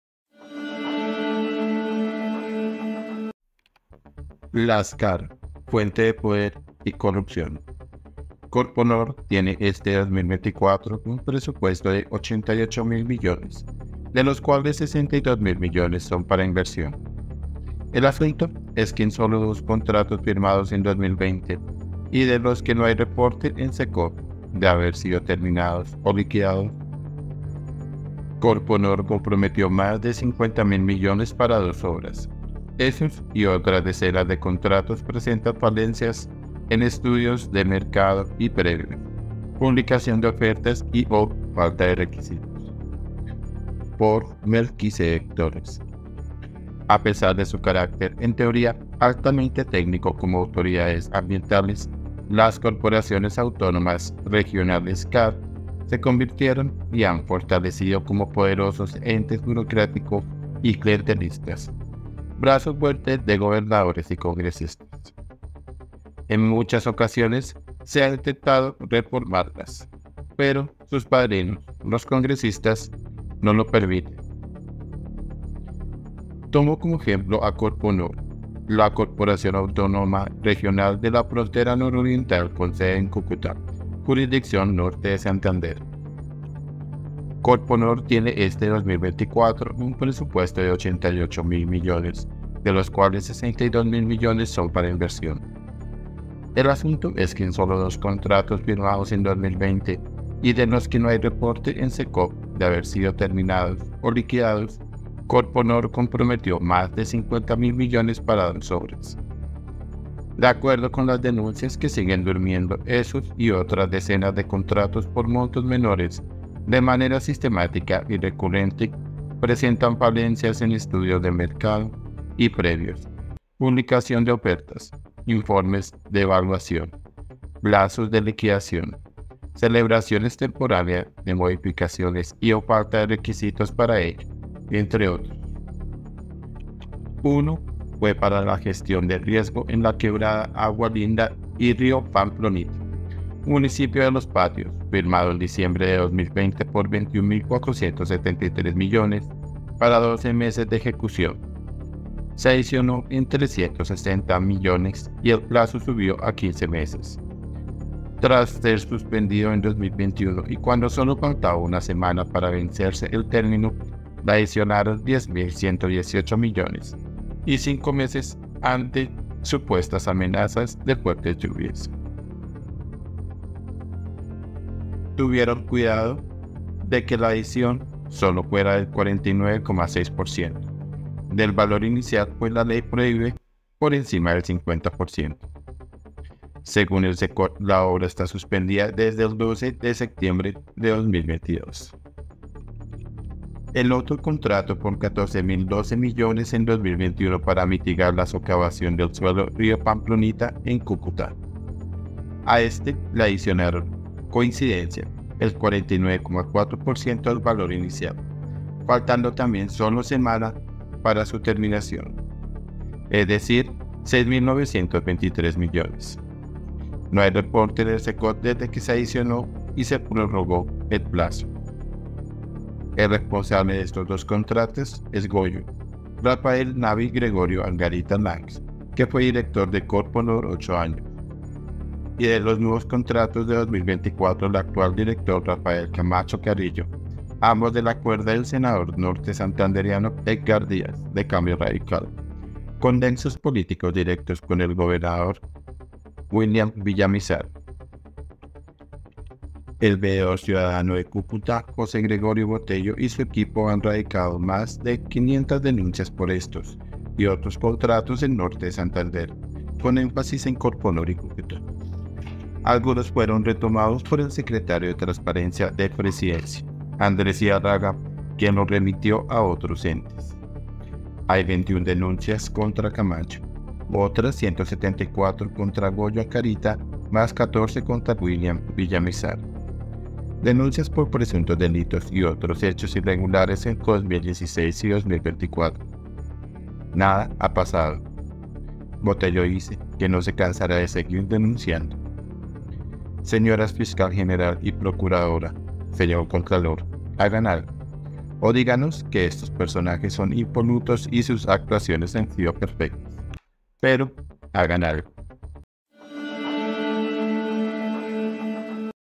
LAS CAR, FUENTE DE PODER Y CORRUPCIÓN ⛔ Por Melquisedec Torres
Corponor tiene este 2024 un presupuesto de $88 mil millones, de los cuales $62 mil millones son para inversión; el asunto es que en solo dos contratos firmados en 2020, y de los que no hay reporte en Secop de haber sido terminados o liquidados, Corponor comprometió más de $50 mil millones para dos obras...esos y otras decenas de contratos presentan falencias en estudios de mercado y previos, publicación de ofertas, y/o falta de requisitos.
columna MELQUISEDEC TORRES